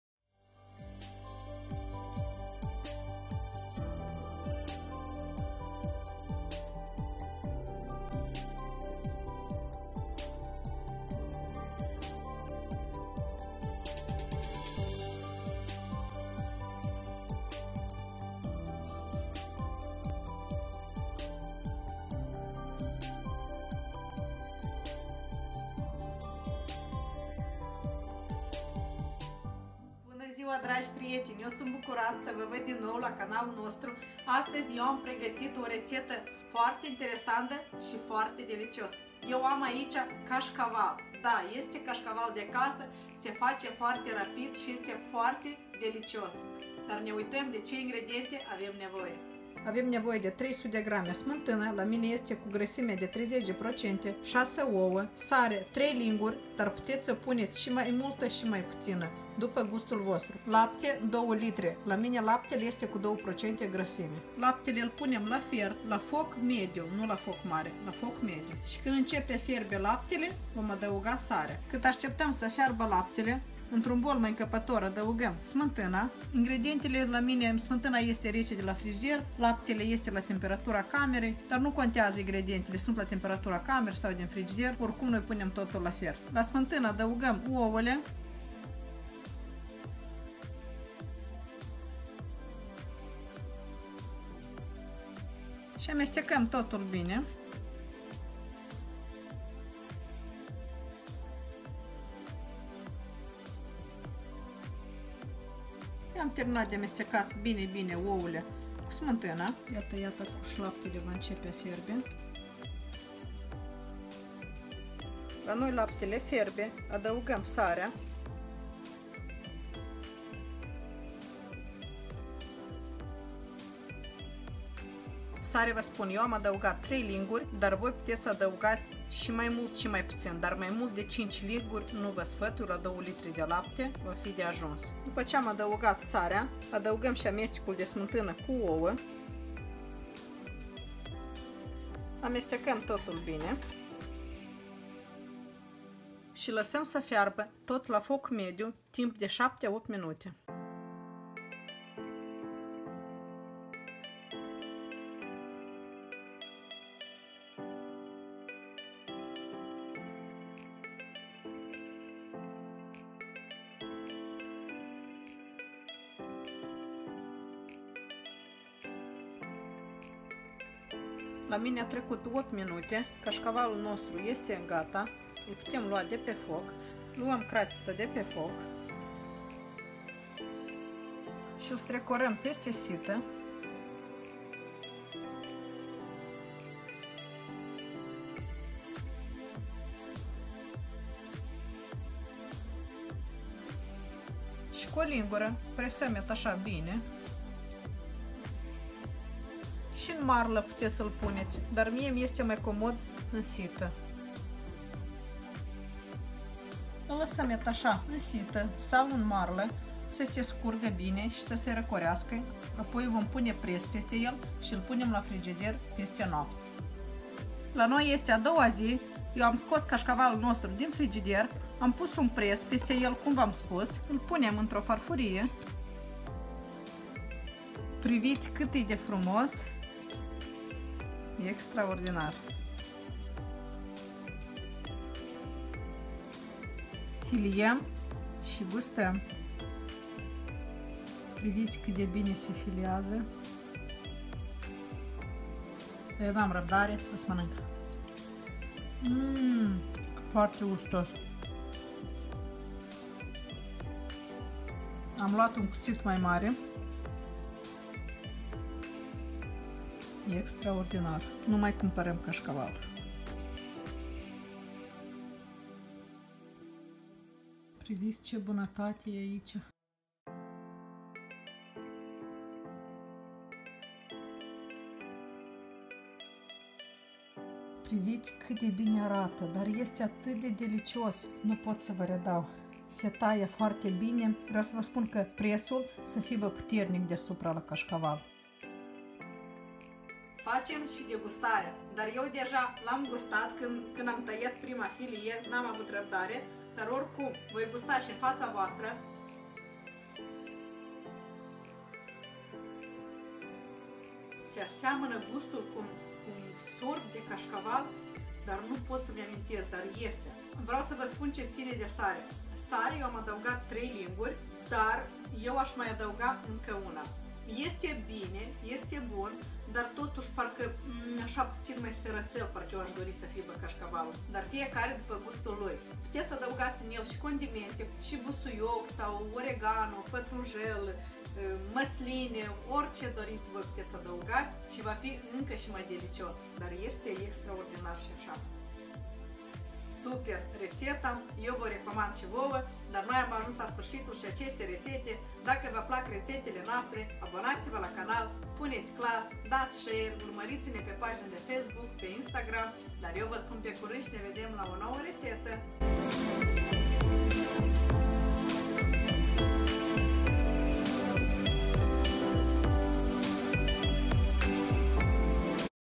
Cascaval in conditii de casa cu produse din comert. UN DELICIU.
Buna Ziua! Aceasta reteta, acest cascaval este minunat delicios si ne costa si mult, mai ieftin, va recomand si voua aceasta bunatate.
Eu va doresc numai bine.